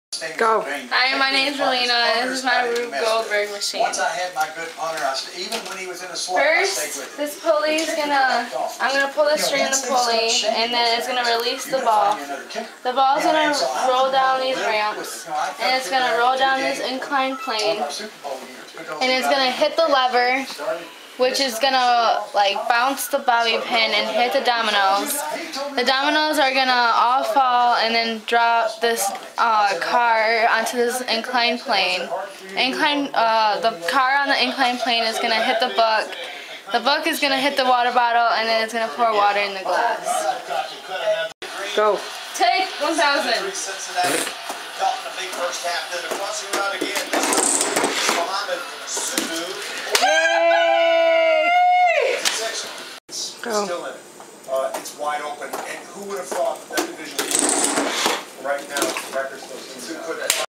Ralina's Rube Goldberg Machine:) Pouring water into a cup
Hi my name is Ralina, and this is my rube goldberg machine. Finally!